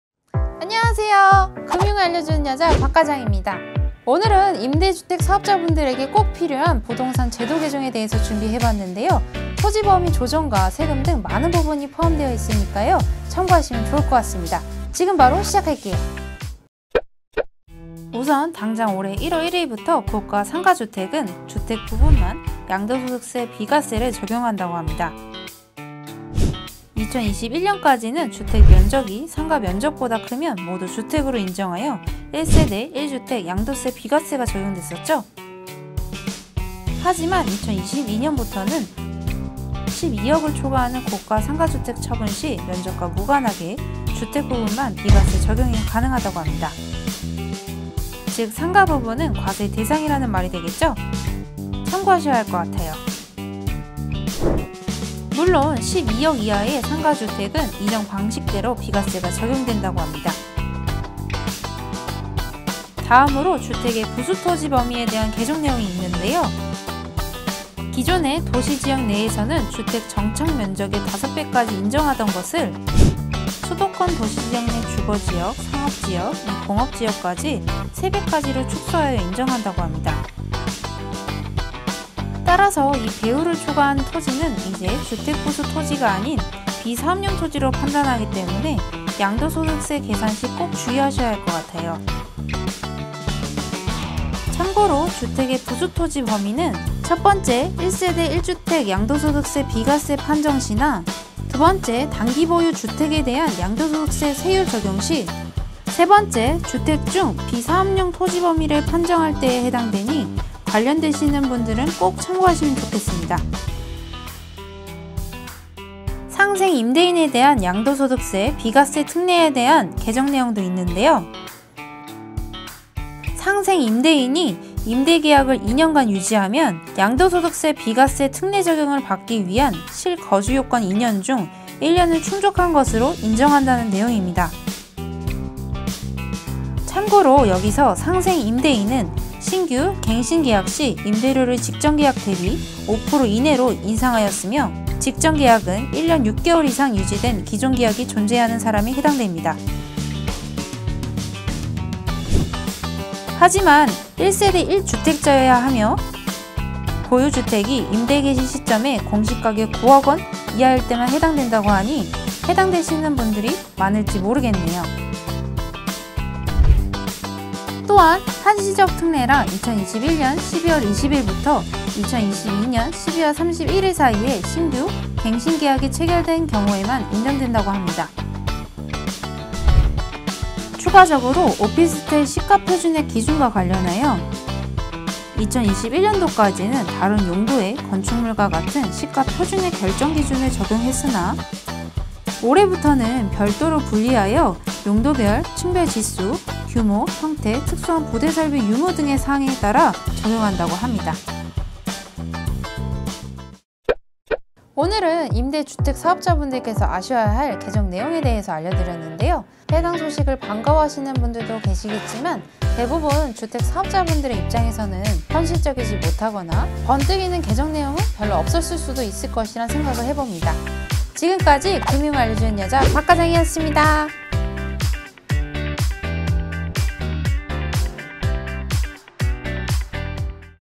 부동산정책! 이렇게 바뀝니다②-주택임대사업자 필수 시청!
지난시간에 청년분들을 위한 부동산정책 개정내용에 대해 다뤄봤는데요!
이번시간에는 반대로 주택임대사업자분들에게 필요한 내용을 준비해봤습니다😀
토지범위조정부터 세금 등 다양한 개정내용이있으니

00:00 - 인트로
00:05 - 오늘의 주제
00:38 - 양도세 관련 개정 내용
01:03 - 부수토지 범위 개정
01:57 - 상생임대인 특례